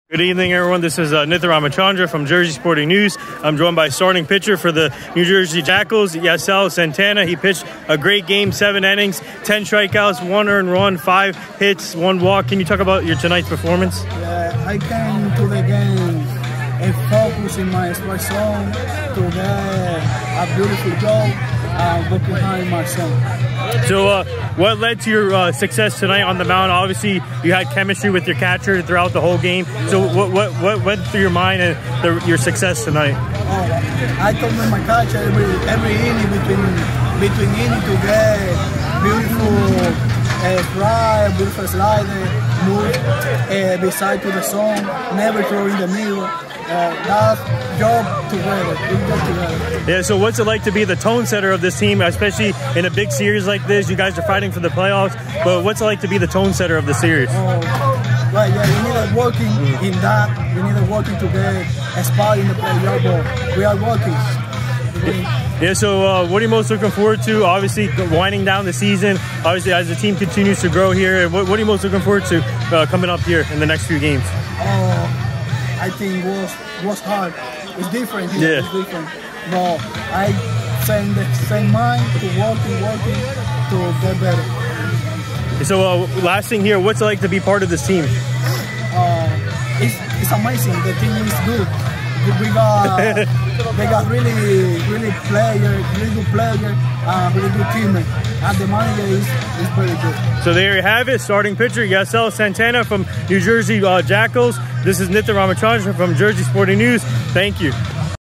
New Jersey Jackals Starter Yasel Santana
Jersey Sporting News Did A Postgame Interview With New Jersey Jackals Starting Pitcher Yasel Santana Yesterday After A Outstanding Performance Going 7 Innings, 5 Hits, 1 Run, 1 ER, 1 Walk And 10 Strikeouts As They Improve To 41-40 💯⚾👍🔥